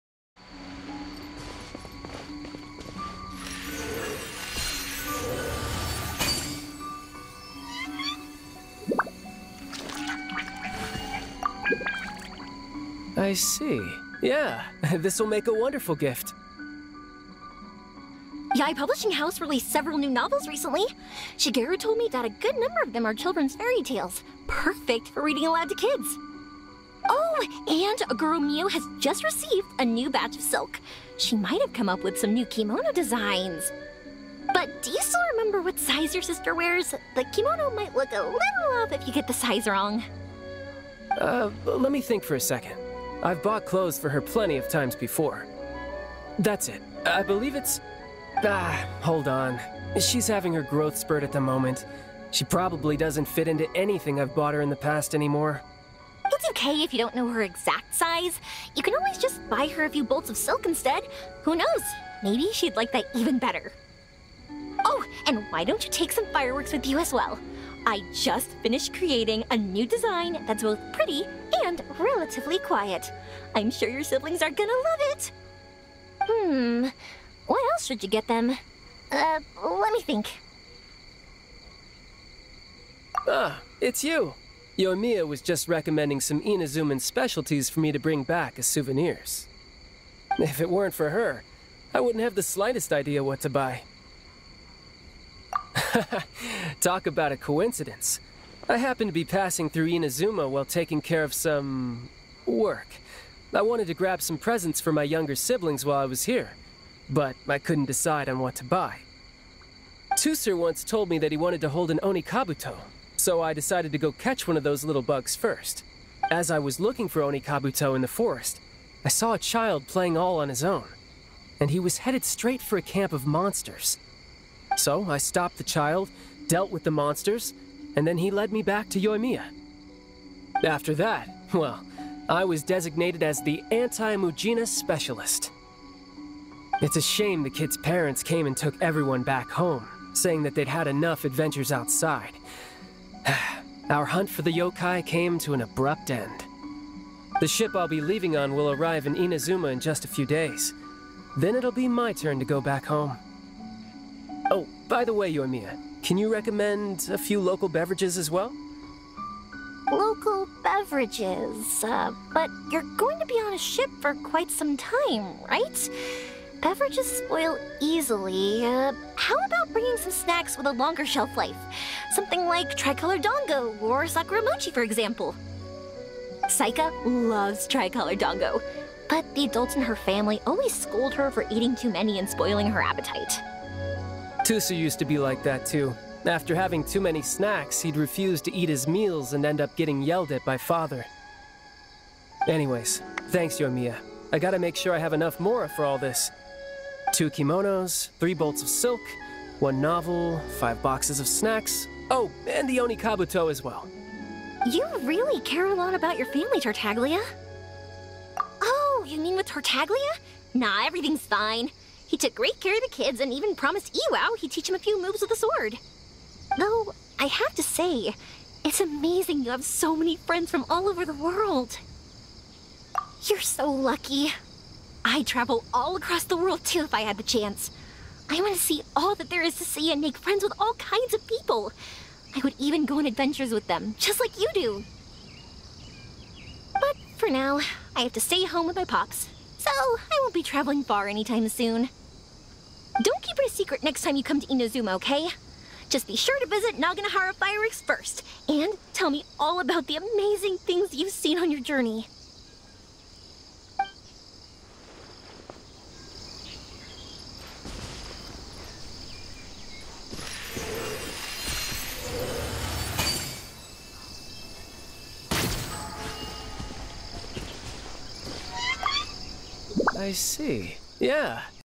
When Tighnari is cautious of the fatui but...
It's TartarGaleria so we give cut him some slack. Still, Tighnari is just cautious.

Anyway, bow threesome amyryte?